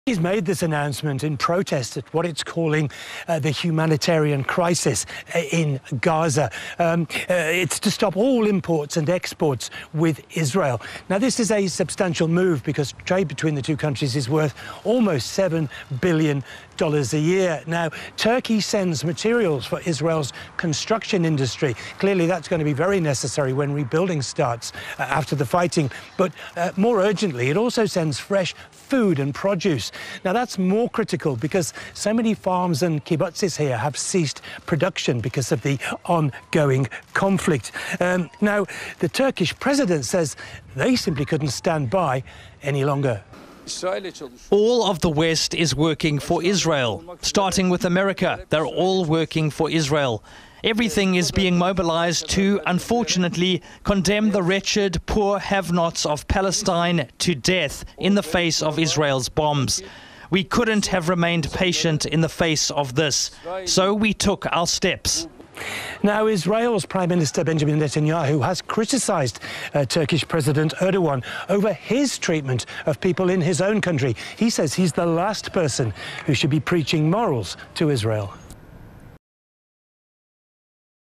Colombia becomes the latest Latin American country to break diplomatic relations with Israel
On Thursday, Colombia joined Bolivia and Belize as the latest Latin American country to break diplomatic relations with Israel, citing Israel's actions in Gaza as "genocide." On Friday, Turkiye, a key partner of Israel, announced its suspension of all trade with Israel.